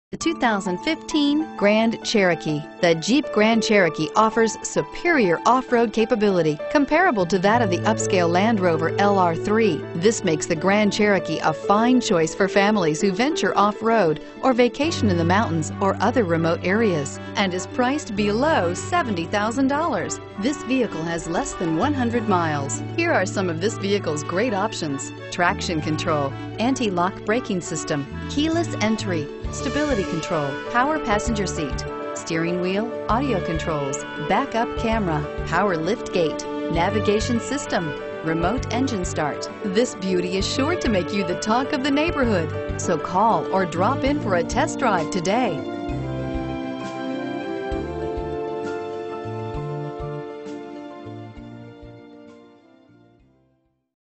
2015 Jeep Grand Cherokee Temple TX FC921766
2015 Jeep Grand Cherokee SRT - $66307

5255 I H 35 South

Pick up this Redline 2 Coat Pearl 2015 Jeep Grand Cherokee, available today at Mac Haik Dodge Chrysler Jeep Ram. Featuring Automatic transmission, it has 11 miles. This could be the one you've been searching for.